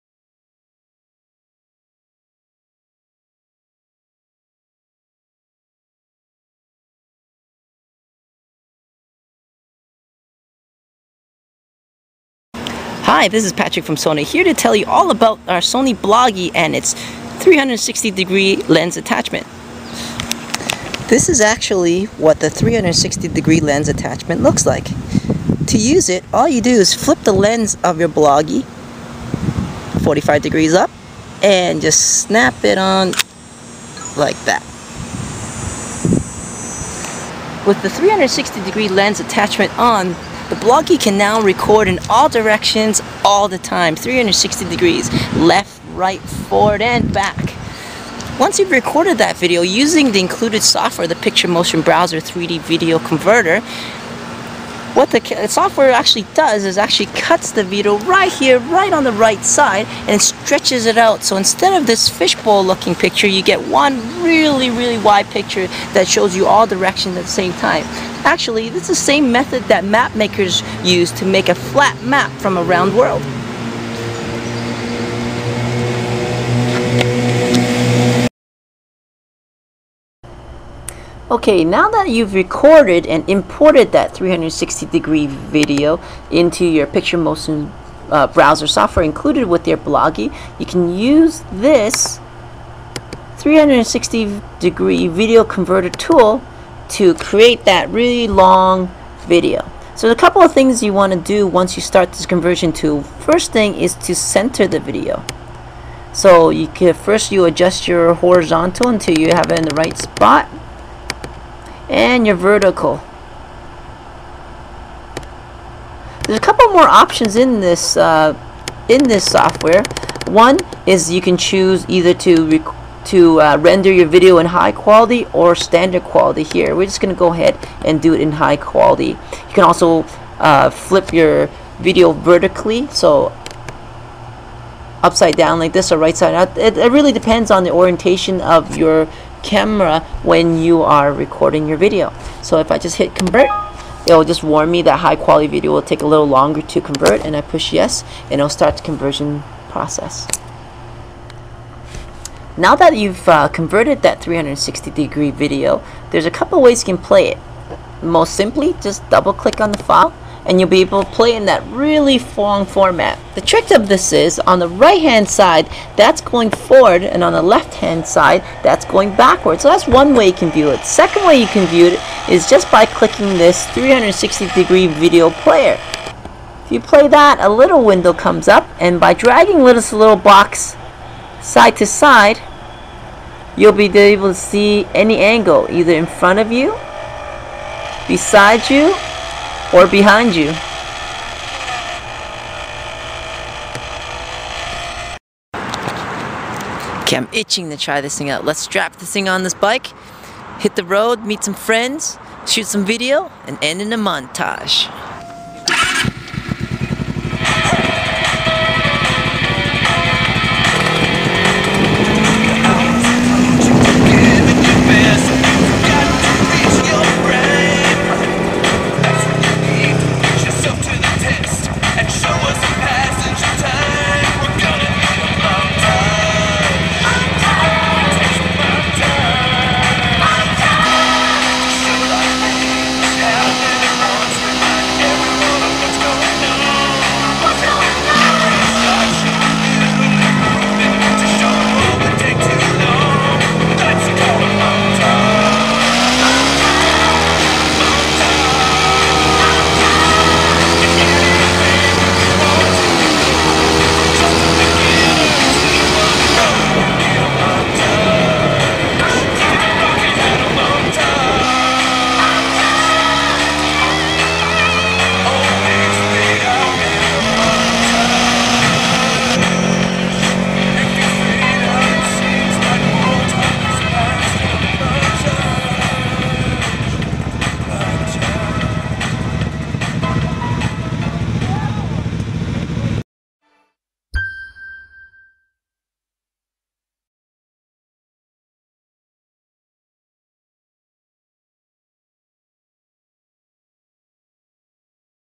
Sony Bloggie 360 Degree Video- How to and Sample Video. -Yoùtùbe Yoùtùbe - HD
What better way to demo the Sony bloggie and its 360 degree lens attachment than mounting it on a bike and riding with friends! Learn how the Sony bloggie ca... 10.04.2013 Technòlògy - Yoùtùbe - HD